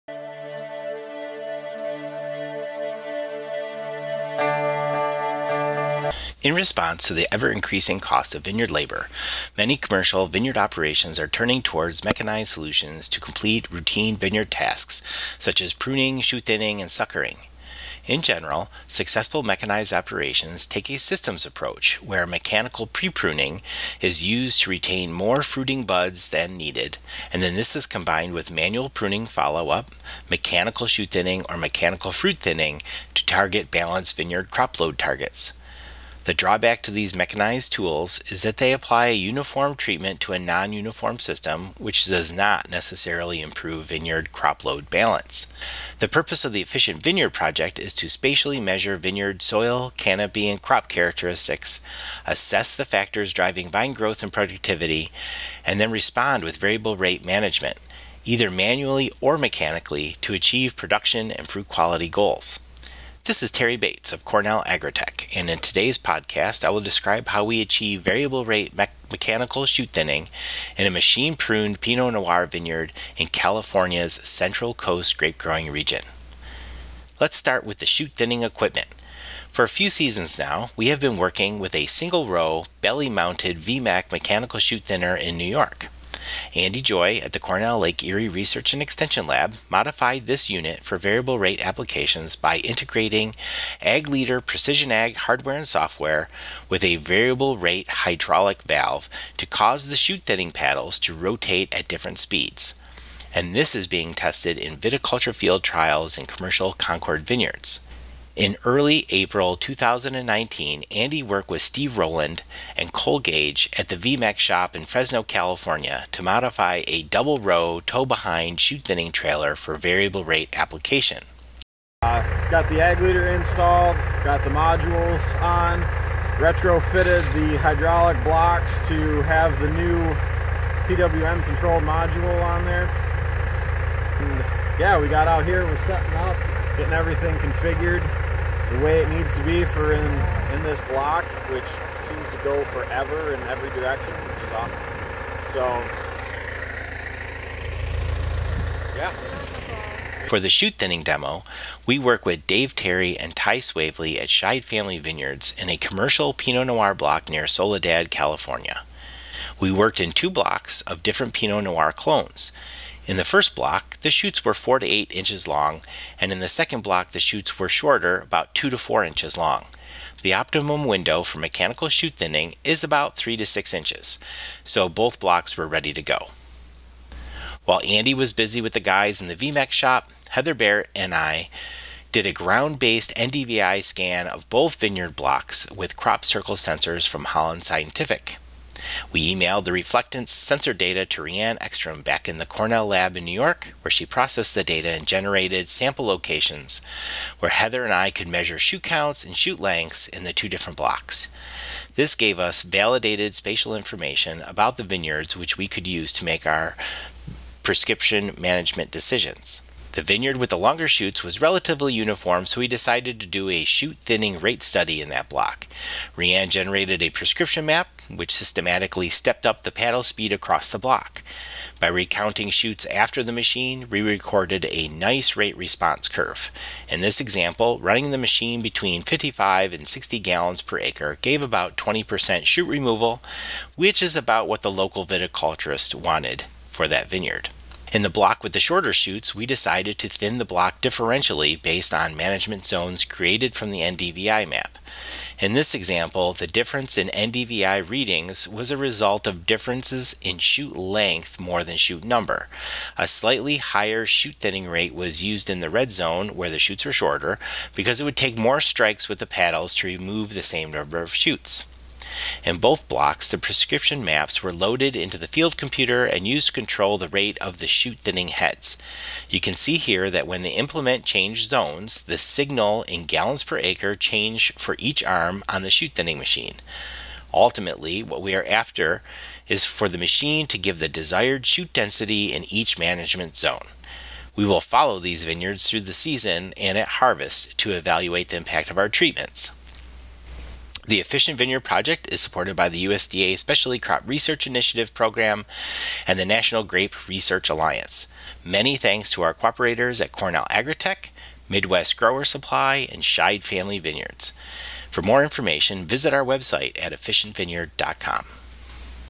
Scheid Shoot Thinning Demo - LERGP Podcast #115
Efficient Vineyard variable rate mechanical shoot thinning in a California Central Coast Pinot Noir vineyard.